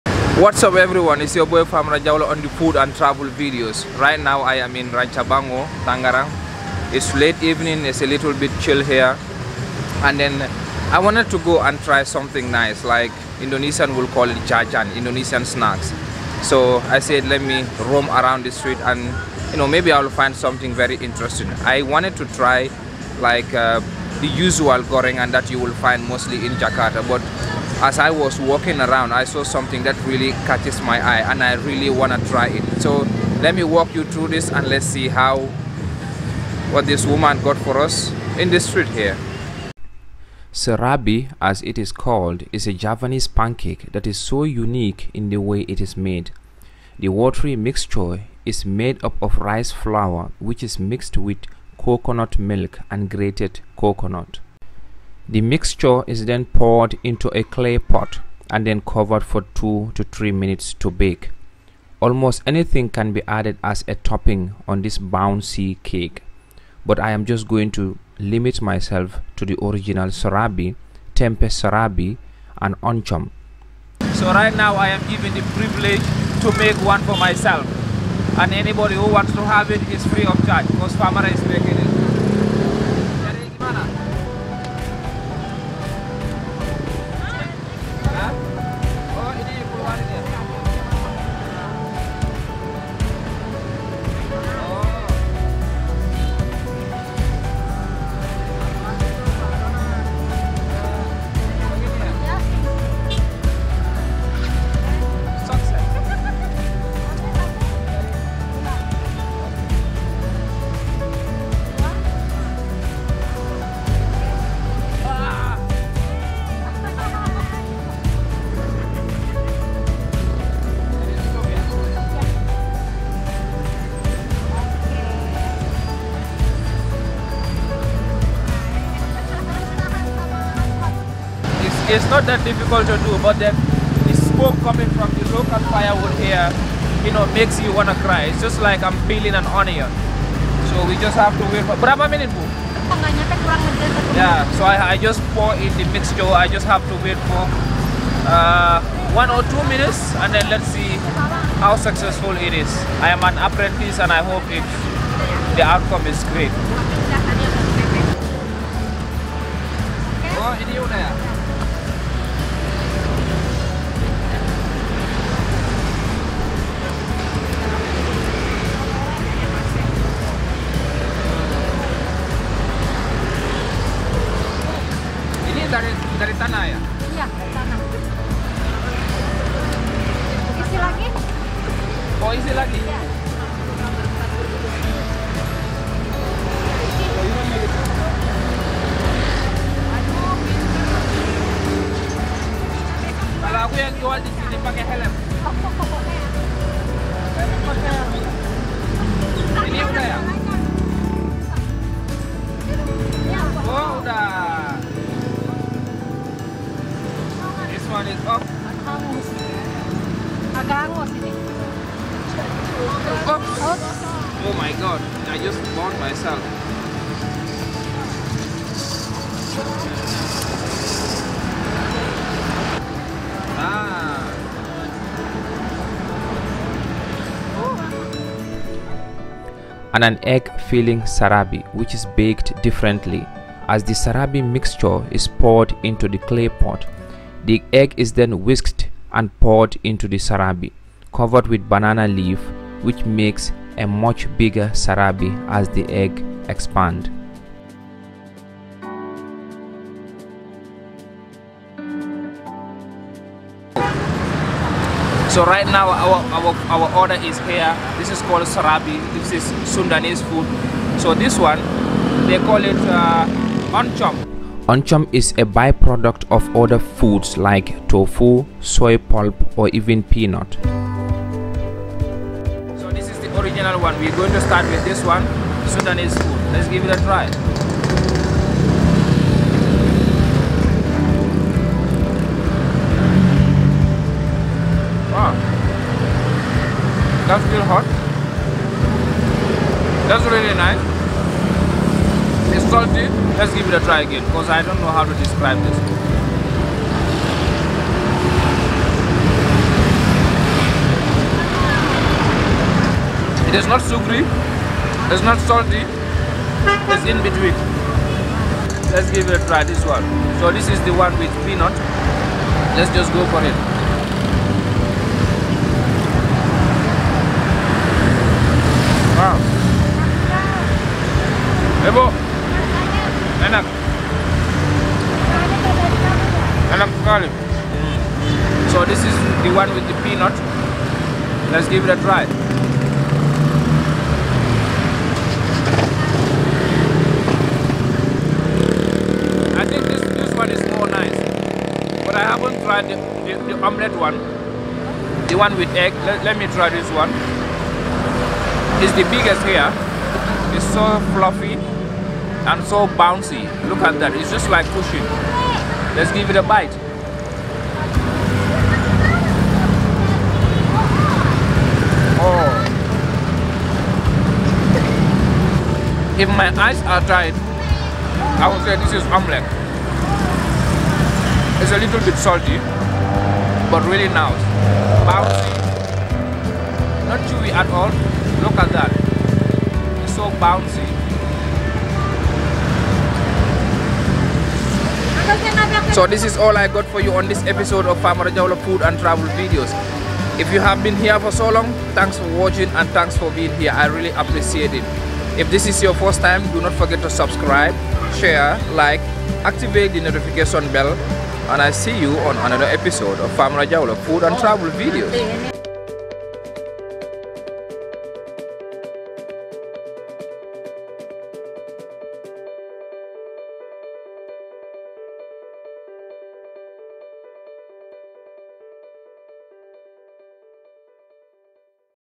Trying and cooking Javanese Serabi - Indonesia street food - Sepatan Part 1
In this video, I tried the famous Indonesia pancake called Serabi in the remote area of Sepatan located in Tangerang.
Serabi is baked differently from other Indonesia Javanese traditional pancakes. it is baked in a clay pot over firewood.
Serabi can have different toppings as the buyer wish and if available. and in this video we tried Tempe Serabi, egg filling Serabi, plain and Oncom topping Serabi.